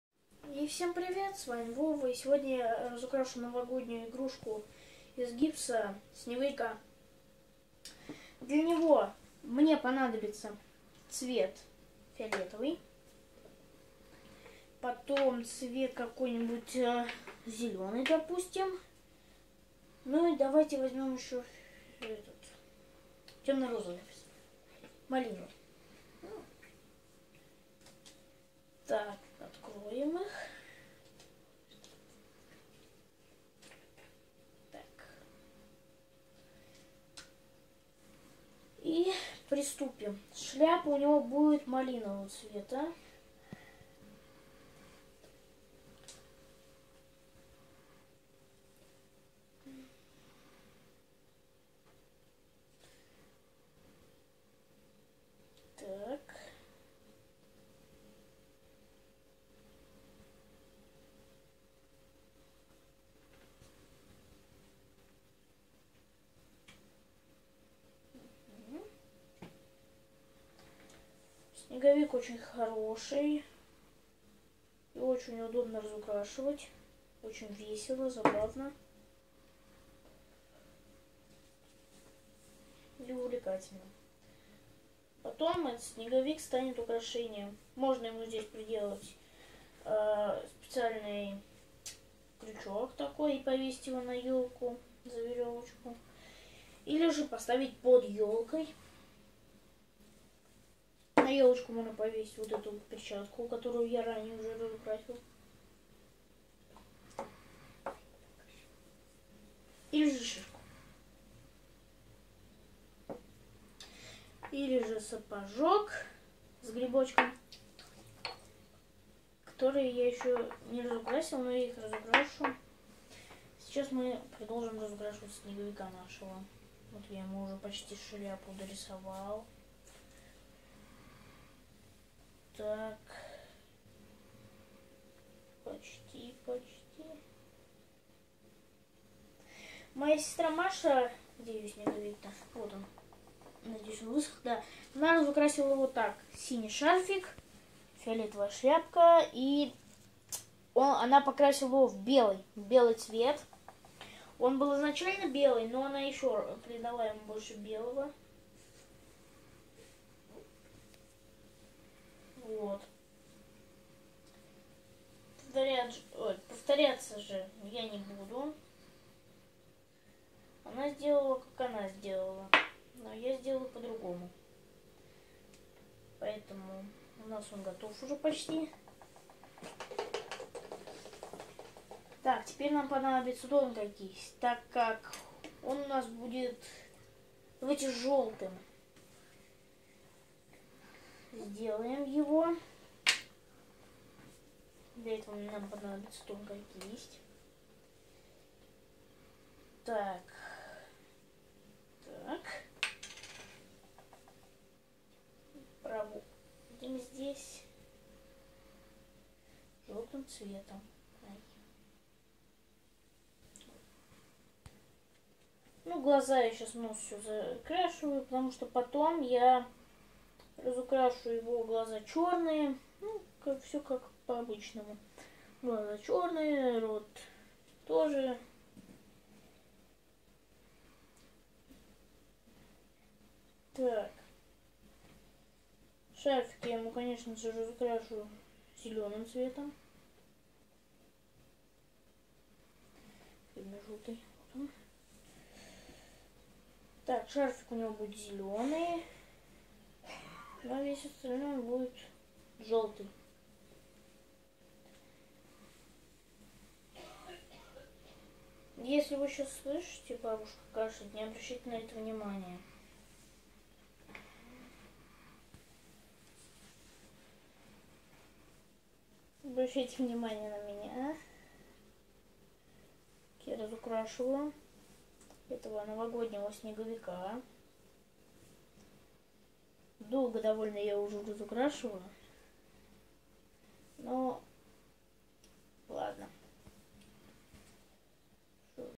Раскрашиваем фигурку снеговика из гипса
Вы можете купить гипсовую фигурку снеговика для своих деток в интернет-магазине "Родные игрушки" Фигурка снеговика одна из самых популярных в новогодней тематике.  Дети любят раскрашивать снеговика, а готовую фигурку можно поставить под елочку.